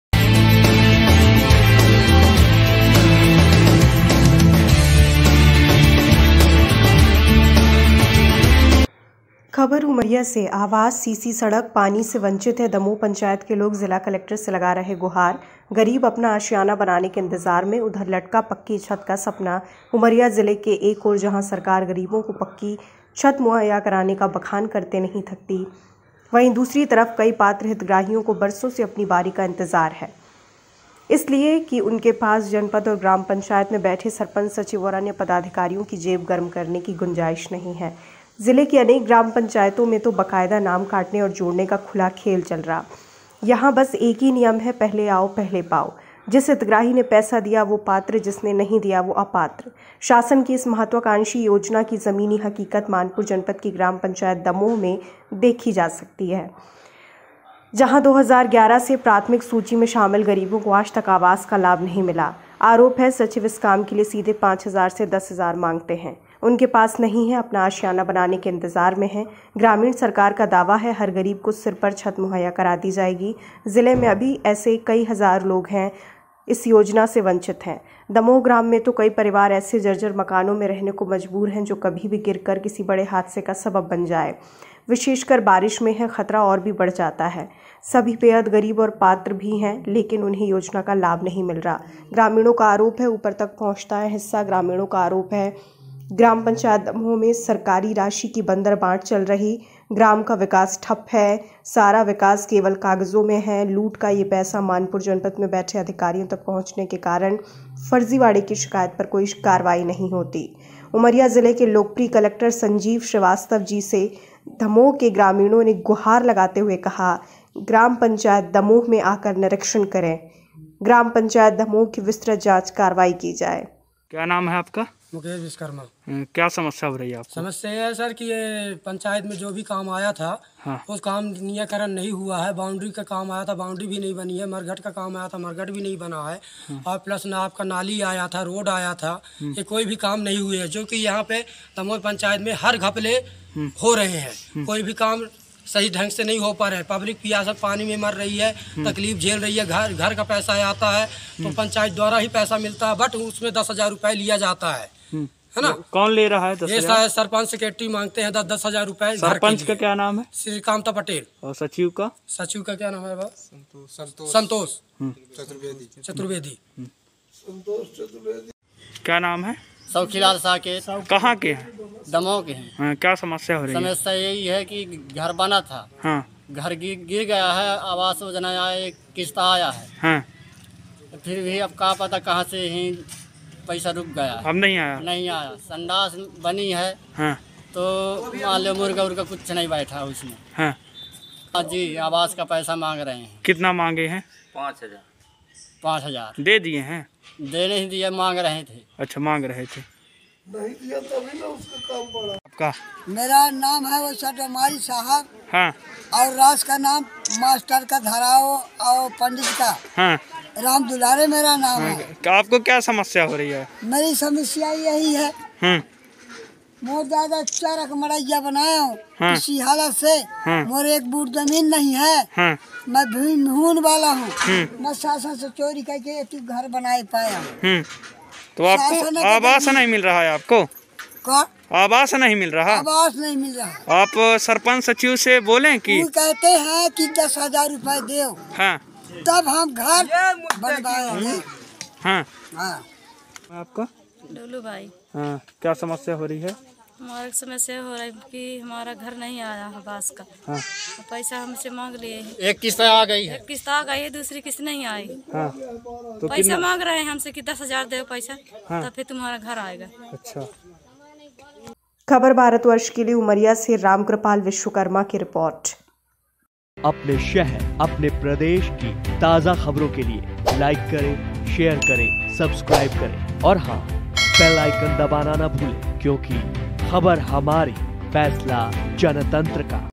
उमरिया : आवास , सीसी सड़क ,पानी, से वंचित है दमोह पंचायत के लोग जिला कलेक्टर से लगा रहे हैं गुहार
गरीब अपना आशियाना बनाने के इंतजार में है, अधर में लटका पक्की छत का सपना
 सड़के और नालियां टूट कर बिखर चुकी हैं सरकारी योजनाओं का ना तो प्रचार होता है और ना ही कोई जानकारी दी जाती है
           कई पात्र हितग्राहियों को बरसों से अपनी बारी का इंतजार करना पड़ रहा है